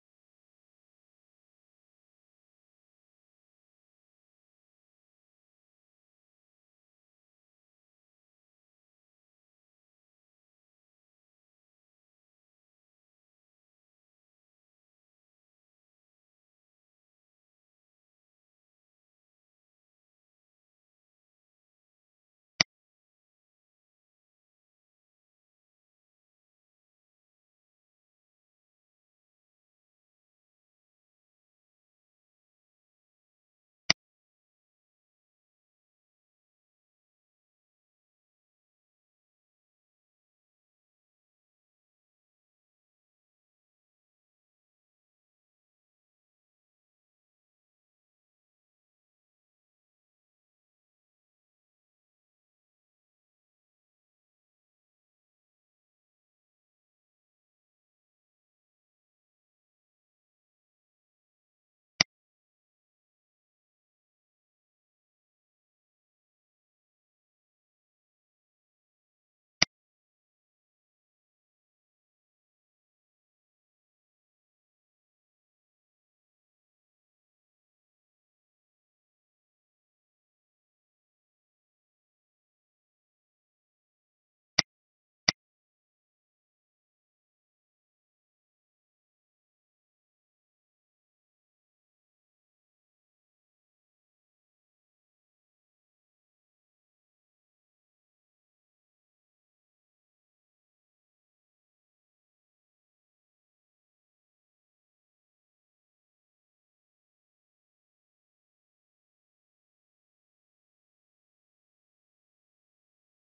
Pourcentage dans les tableaux croisés dynamiques Excel - Formation Excel Marseille
Tutoriel vidéo sur la fonctionnalité d'Excel : Calcul des résultats d'un tableau croisé dynamique en pourcentage issu de la formation Excel perfectionnement de Optivalys Formations à Marseille.